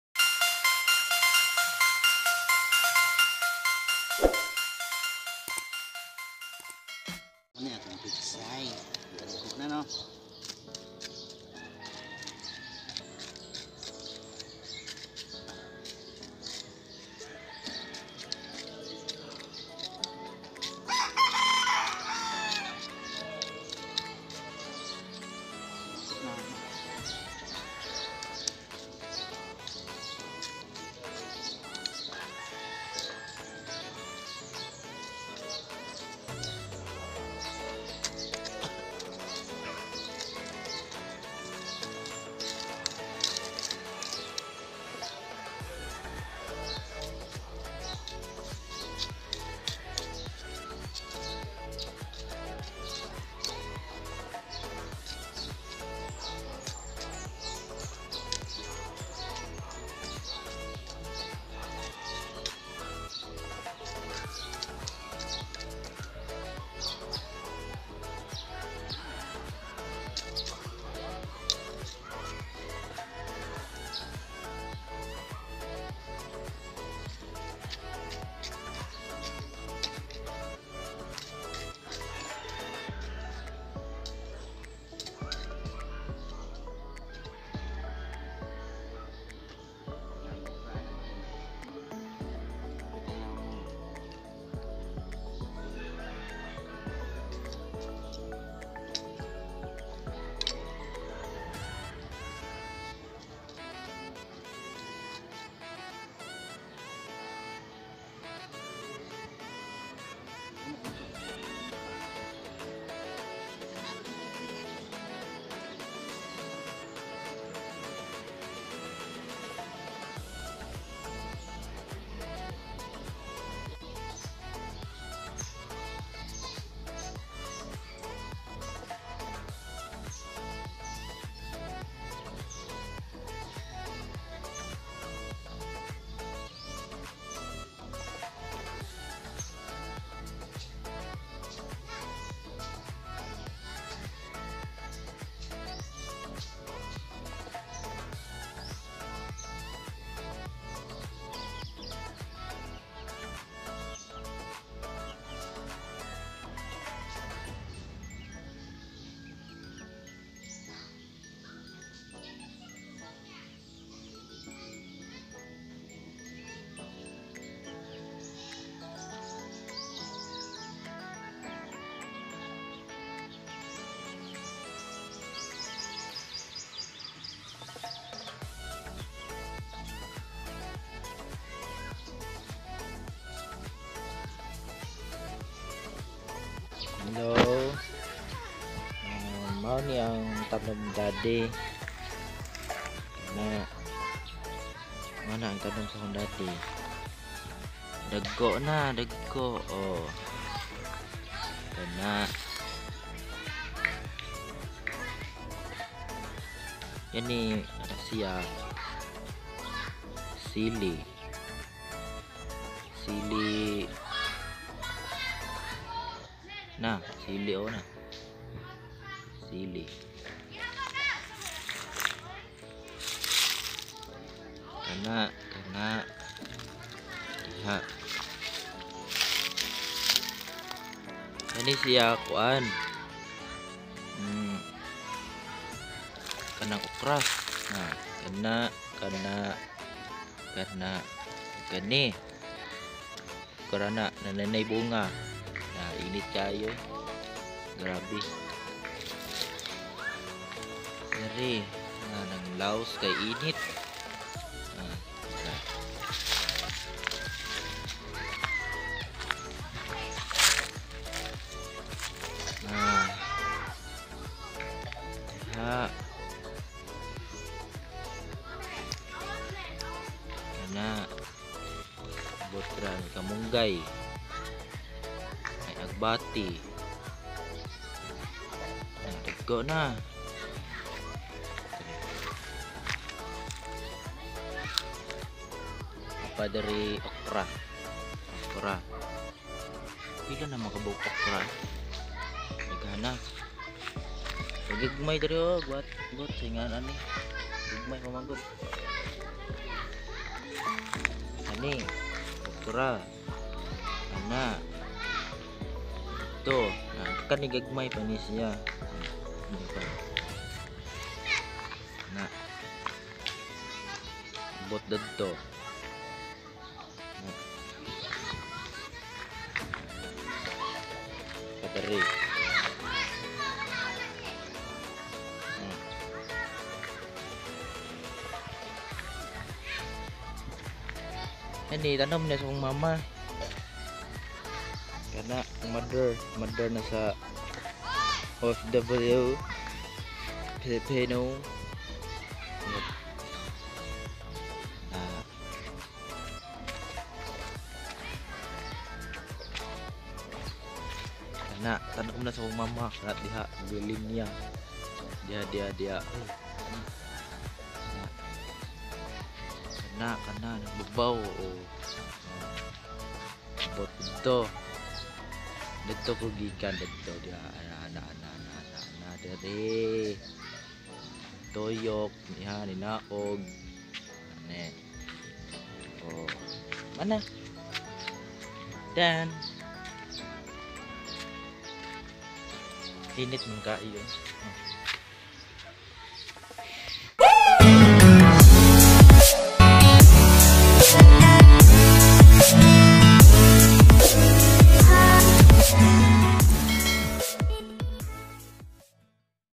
Maliit na Garden,Anong Meron.
May sili,pipino, alogbati,okra at
Malonggay.